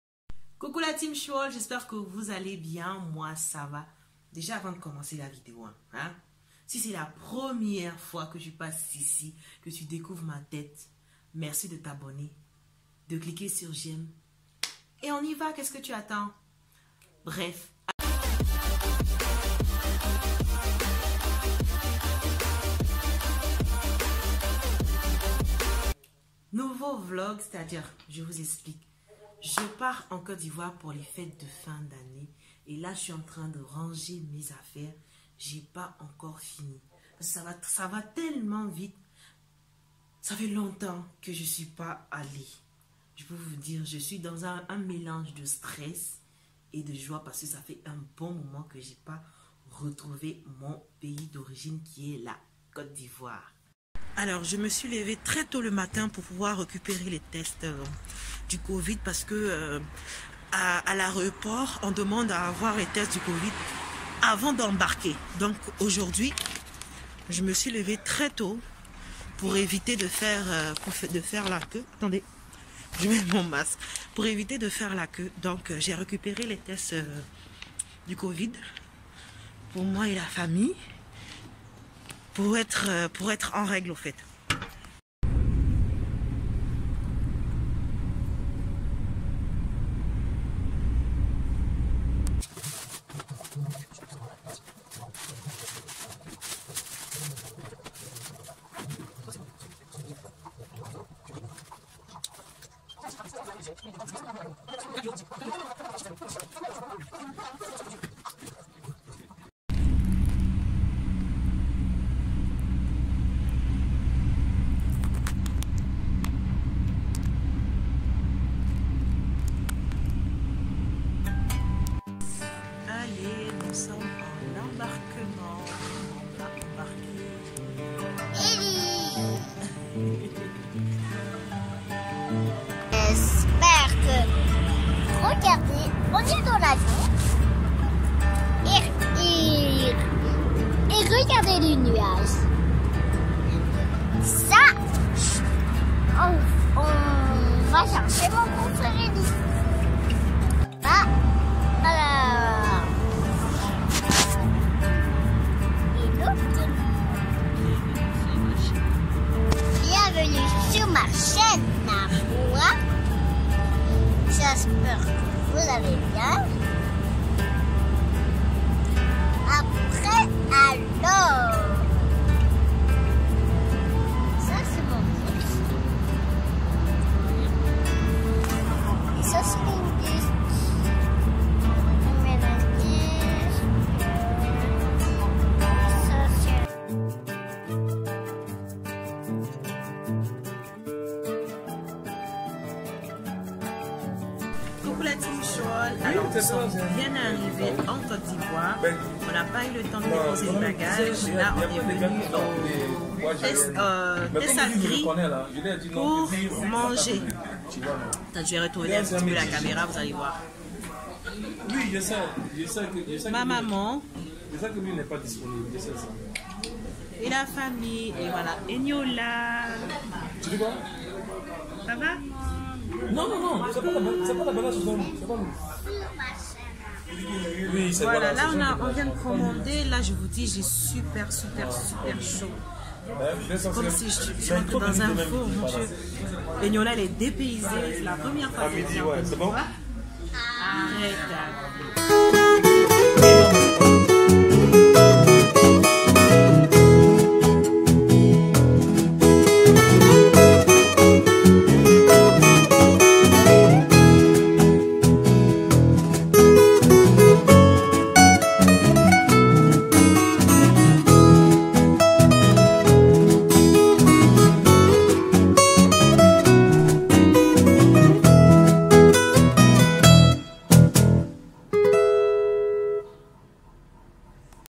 [Vlog] Côte d'Ivoire mon départ pour Abidjan
Très heureuse d'être en Côte d'Ivoire pour les vacances de fin d'année je vous montre les préparatifs de mon voyage et mon arrivée 😊